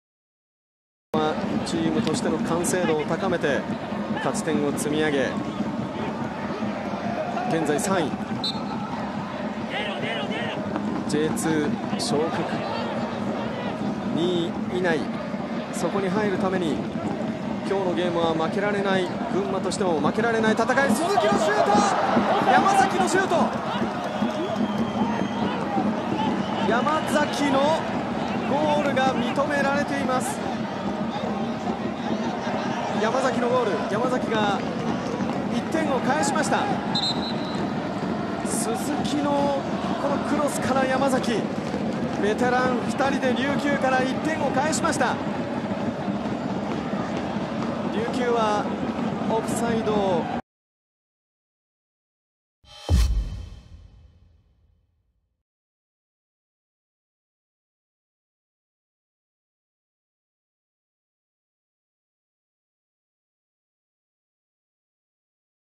【公式】ゴール動画：山﨑 雅人（群馬）61分 ＦＣ琉球vsザスパクサツ群馬 明治安田生命Ｊ３リーグ 第30節 2018/11/3
左クロスからゴール前に入った山﨑 雅人（群馬）が相手GKのタイミングを外す形でシュート！群馬が１点を返す！明治安田Ｊ３のゴール動画を公開中！
2018年11月3日（土）に行われた明治安田生命Ｊ３リーグ 第30節 琉球vs群馬のゴール動画です。